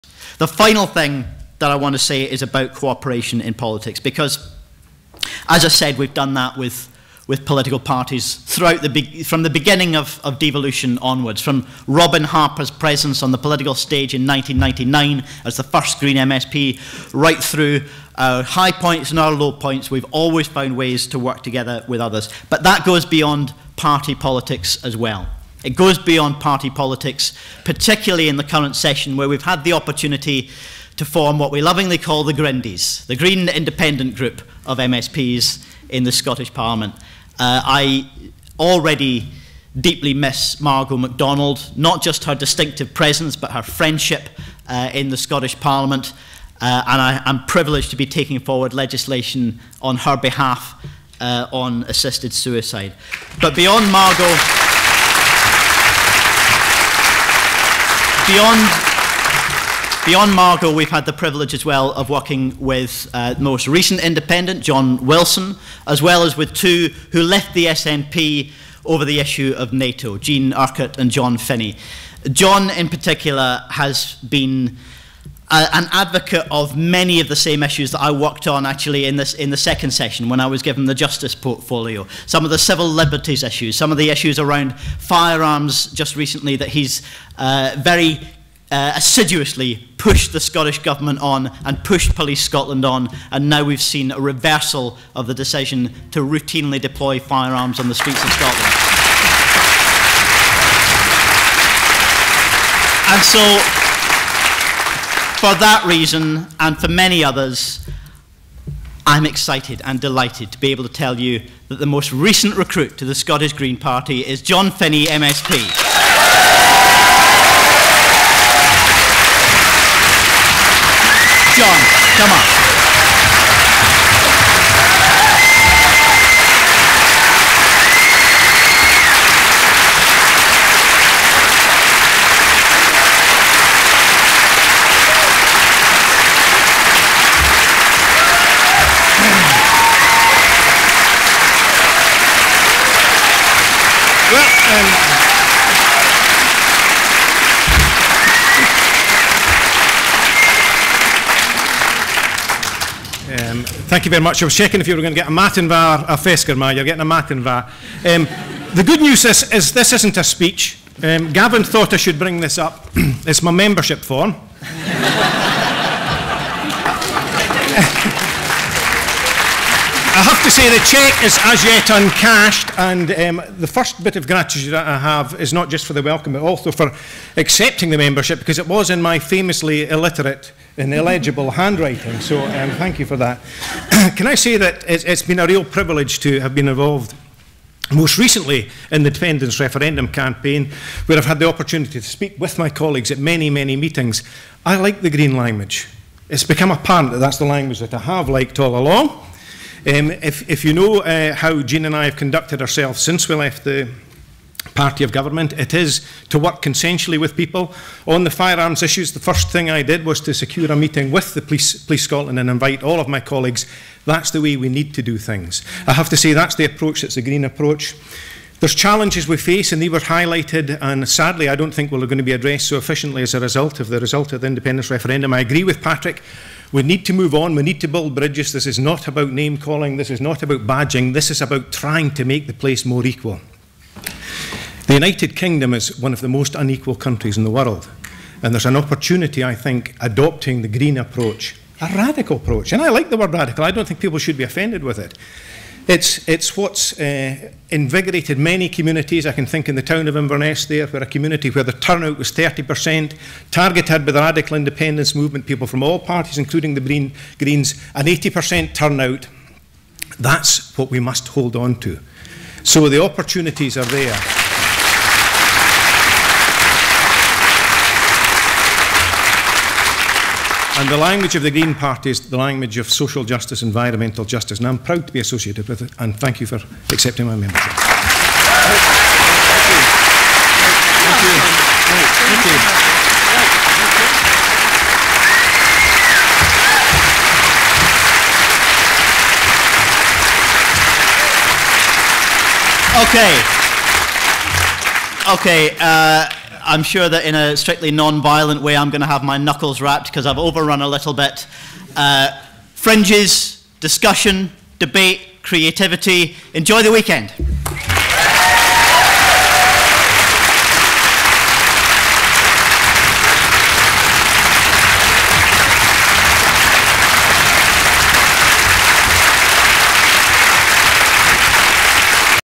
John Finnie, Highlands & Islands MSP joins the Greens
John Finnie, independent MSP for the Highlands and Island, has joined the Scottish Green Party. He was welcomed to the party with a standing ovation during Patrick Harvie MSP's speech to the Green conference at Napier University in Edinburgh.

"I've been a Green all my life; I just didn't know it. Working and campaigning alongside Greens, I've seen that my values are Green values: social and environmental justice, democracy and integrity, internationalism and peace. Receiving my membership card today feels like a homecoming."